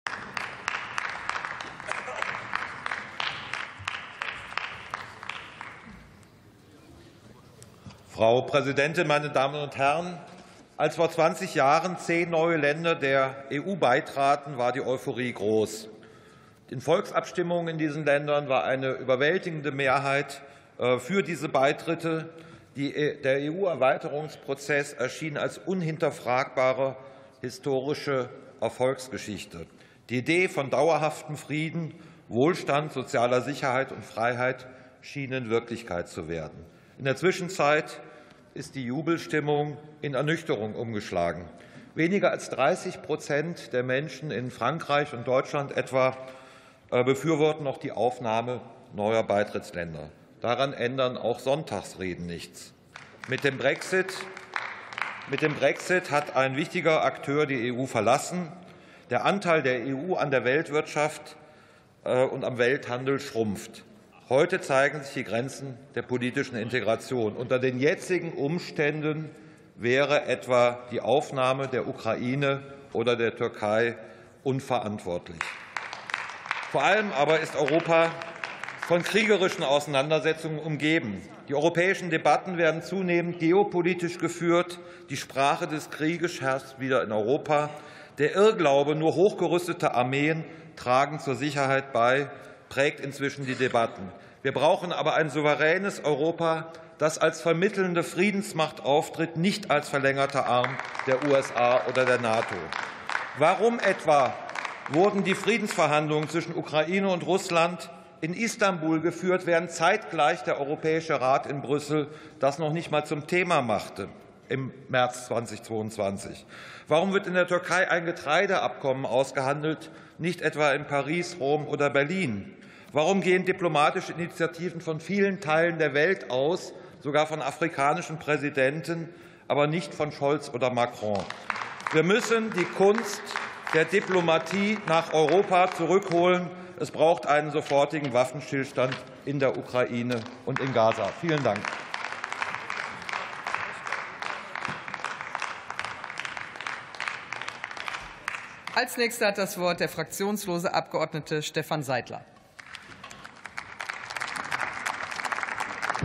Die Kunst der Diplomatie nach Europa zurückholen
Rede in der Debatte zum Thema „20 Jahre EU-Osterweiterung“.